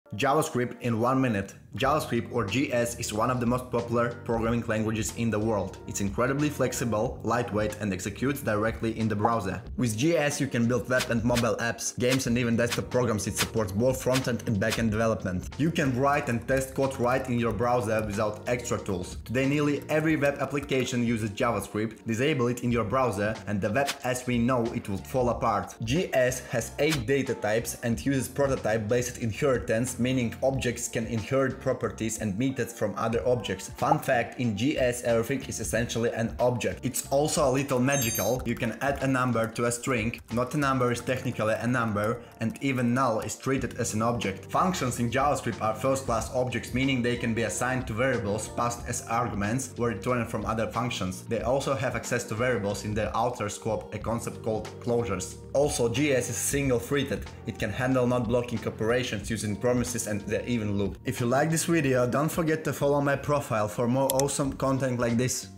"JavaScript in 1 Minute: What Makes It So Powerful? 🚀"
- JavaScript has 8 data types you need to master.

- Functions in JavaScript are first-class objects—you can store them in variables, pass them as arguments, or even return them from other functions! 🤯

- And the best part? You can start coding directly in your browser! 🌐

Whether you're just starting out or brushing up on the basics, this reel has you covered. Watch now and unlock the secrets of JavaScript! 💻✨

👉 What’s your favorite of JavaScript? Drop it in the comments!
🙌 Don’t forget to follow @daniel.webdev for more bite-sized coding tips and tricks.